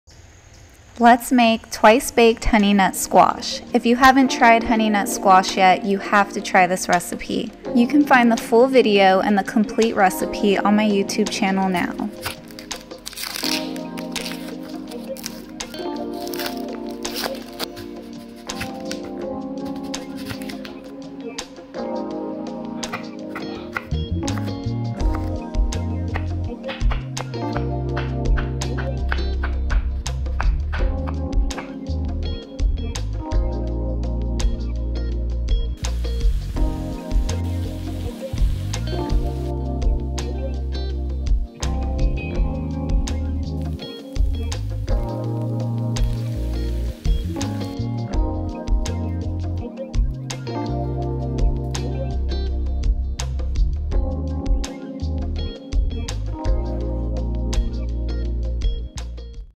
Honeynut Squash- So Underrated!
Twice Baked Honeynut Squash. SO GOOD! Head over to my channel for the full recipe and let me know if you give it a try :)